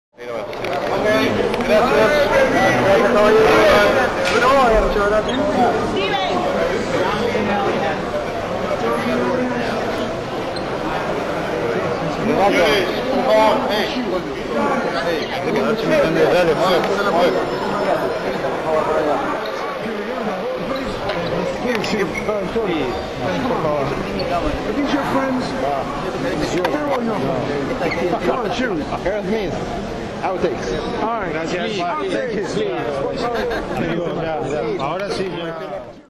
Steven Tyler @ Marriott Hotel, Lima, Peru 21 May 2010
Steven Tyler meeting fans and saying "outtakes".
Marriott Hotel, Lima, Peru 21 May 2010

Everything there is to know about Aerosmith Outtakes, Demos and Rare songs can be found @